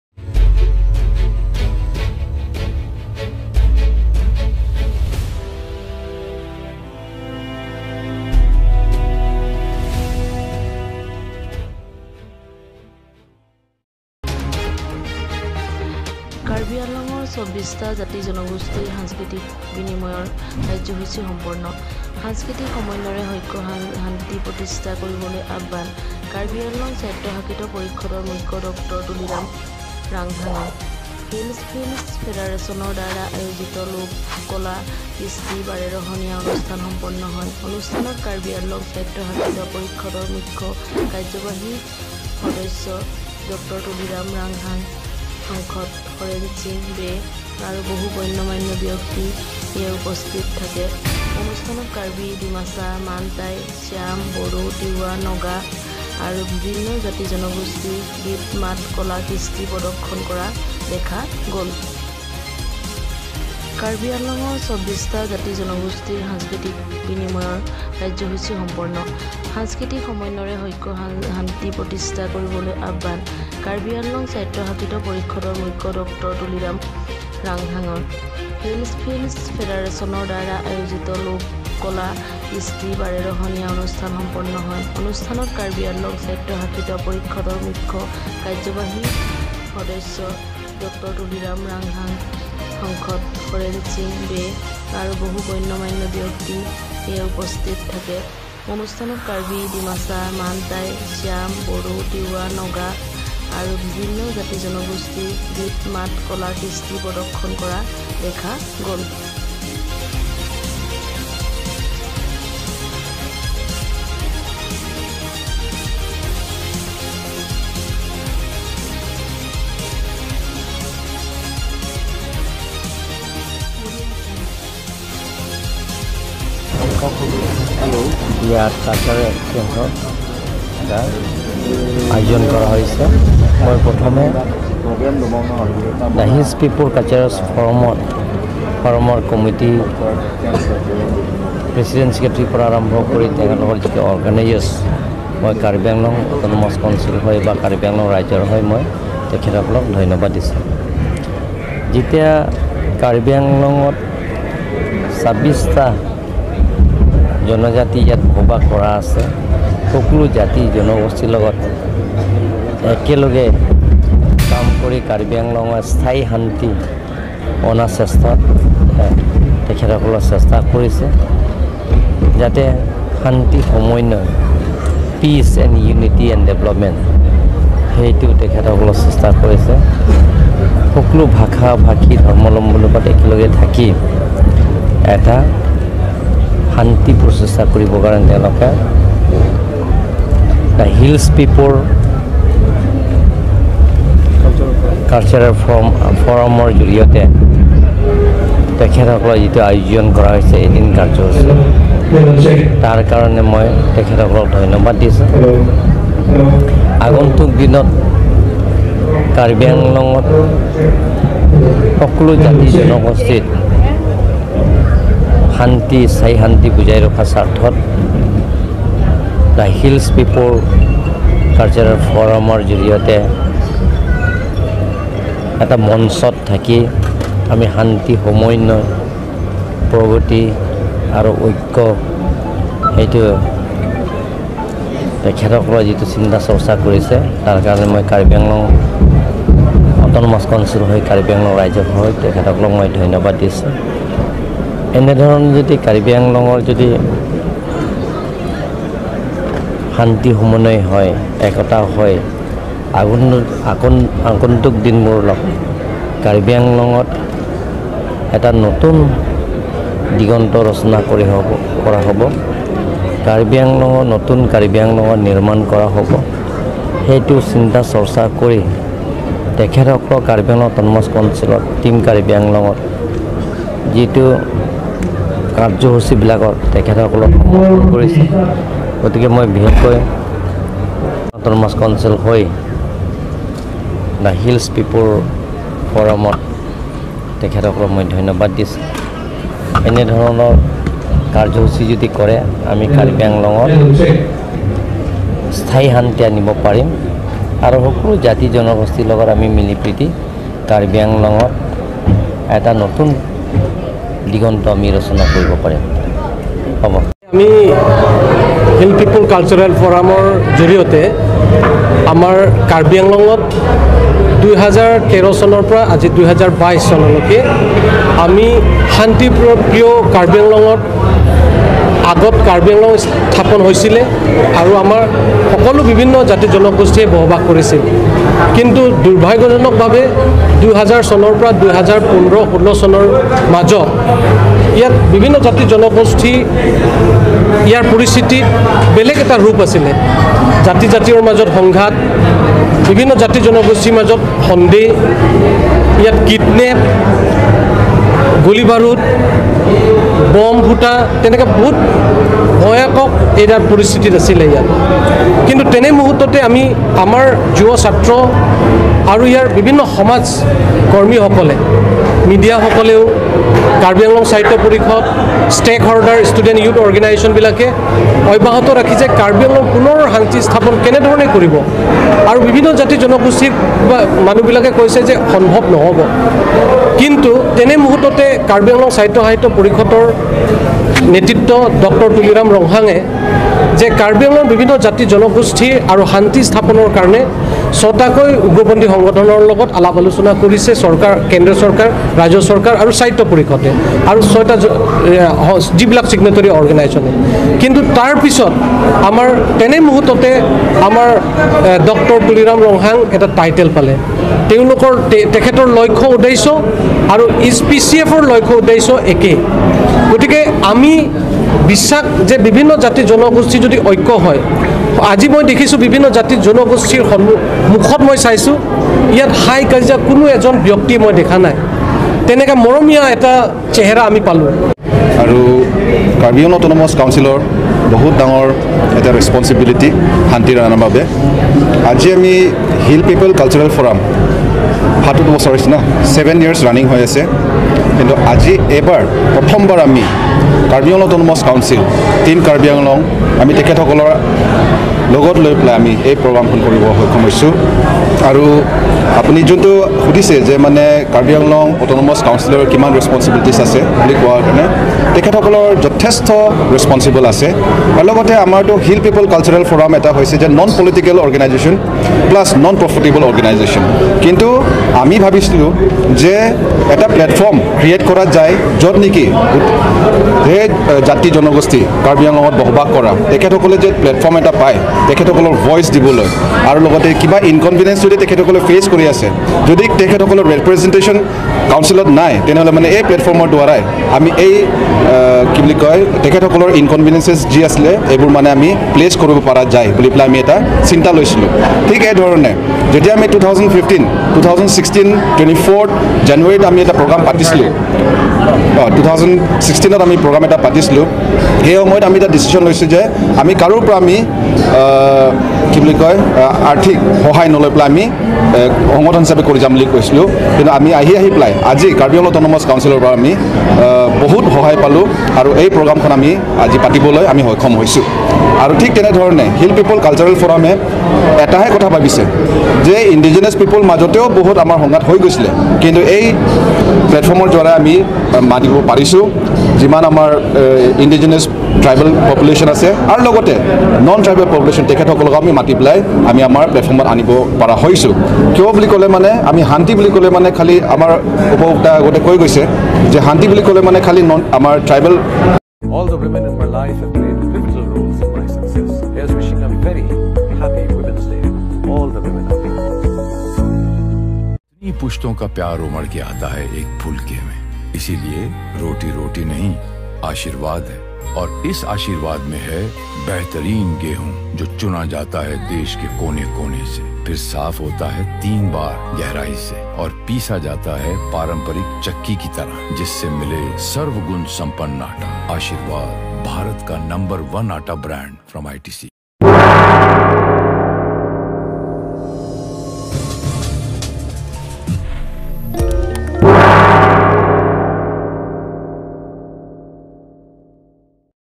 KAAC CEM Dr Tuliram Ronghang on Hills People's Cultural Meet সাংস্কৃতিক বিনিময়ত ড° তুলিৰাম ৰংহাং
KAAC CEM Dr Tuliram Ronghang on Hills People's Cultural Meet 2022 কাৰ্বি আংলঙৰ ২৪ জাতি জনগোষ্ঠীৰ সাংস্কৃতিক বিনিময় অনুষ্ঠানত ড° তুলিৰাম ৰংহাং